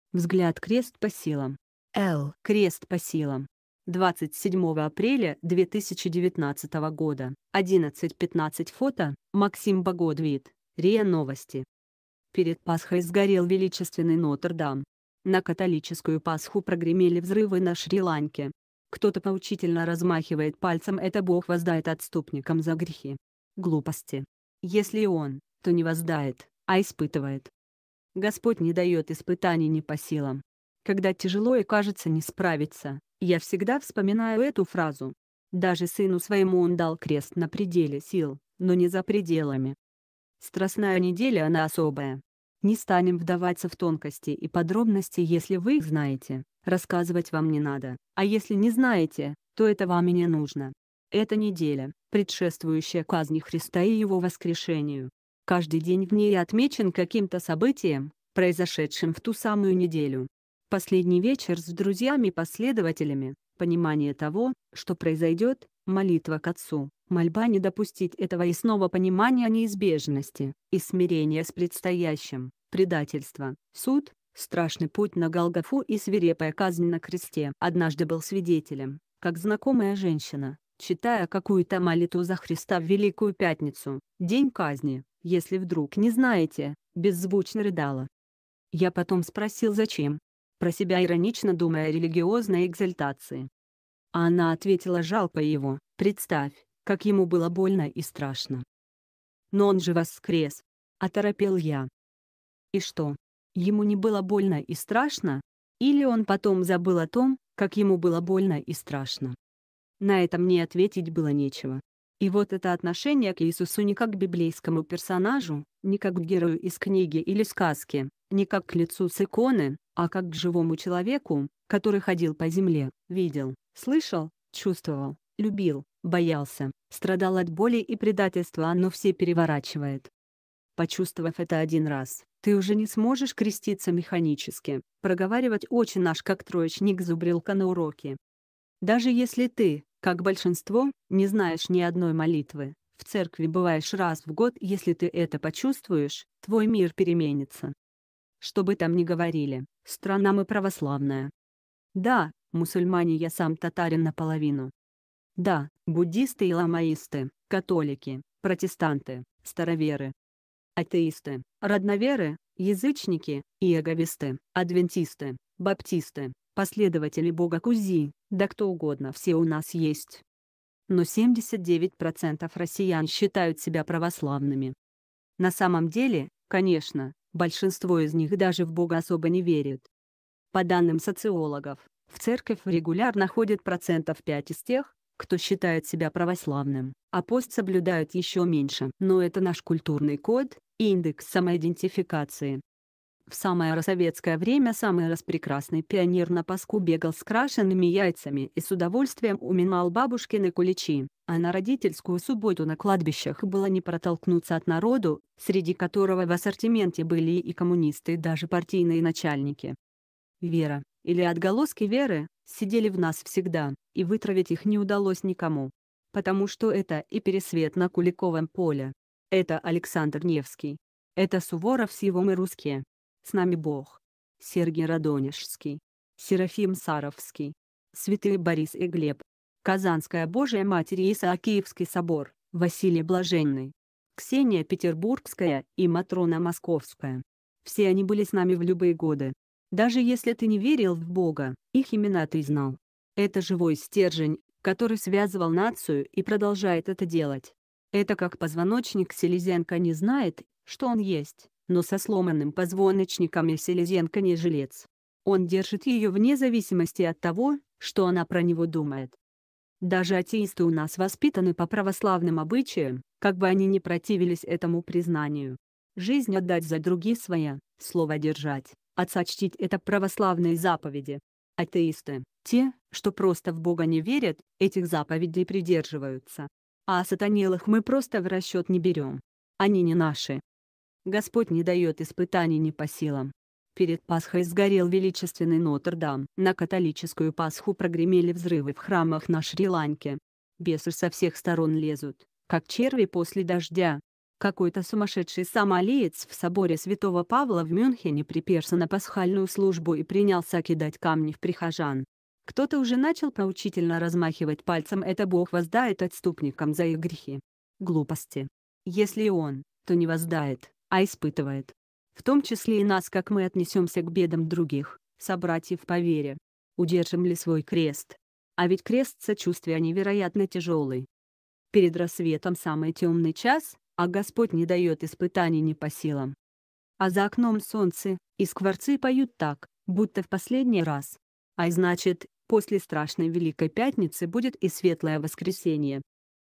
ВЗГЛЯД Крест по силам
ВЗГЛЯД Крест по силам l Крест по силам 27 апреля 2019, 11 15 Фото Максим Богодвид РИА Новости Перед Пасхой сгорел величественный Нотр-Дам . На католическую Пасху прогремели взрывы на Шри-Ланке . Кто-то поучительно размахивает пальцем это Бог воздает отступникам за грехи . Глупости . Если и Он, то не воздает, а испытывает . Господь не дает испытаний не по силам . Когда тяжело и кажется не справиться, я всегда вспоминаю эту фразу . Даже Сыну своему он дал крест на пределе сил, но не за предела